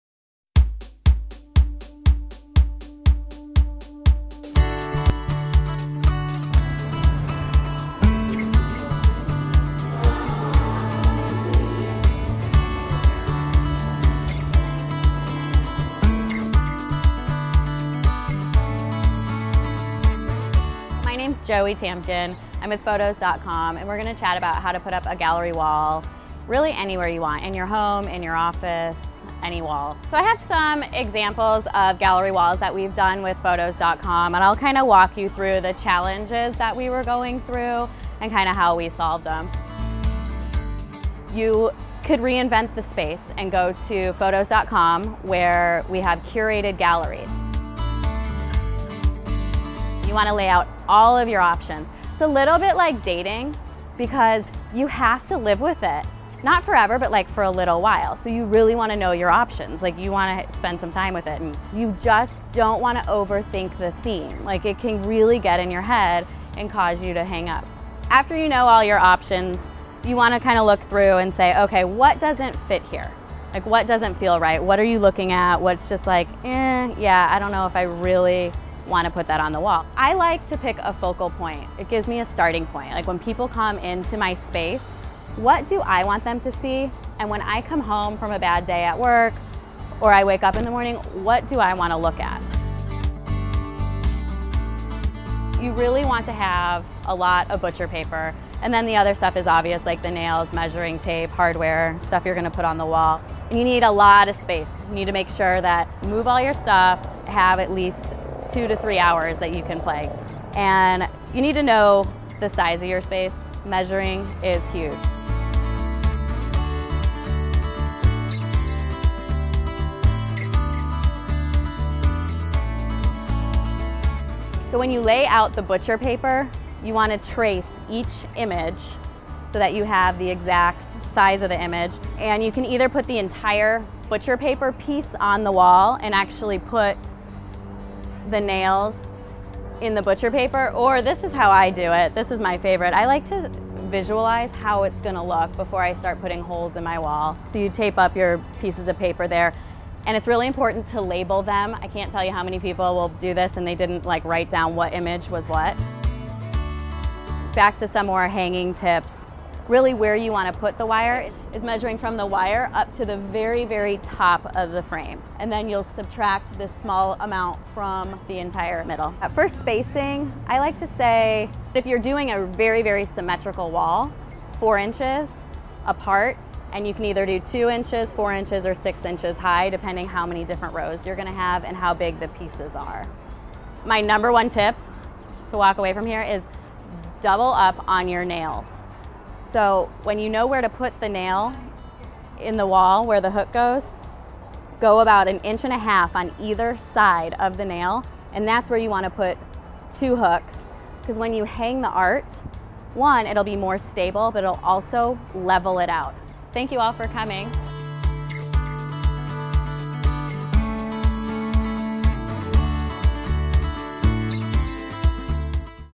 Tips for Decorating your Walls with Framed Photography
Wall Décor Designer, Joie Tamkin, shares tips on how you can create a professional gallery wall on your own. Learn how to mix and match photography with art, black-and-white with color, and different types of frames and materials to give your walls a unique look with your personal touch. Joie teaches you simple tricks to hang your gallery wall like a professional art installer.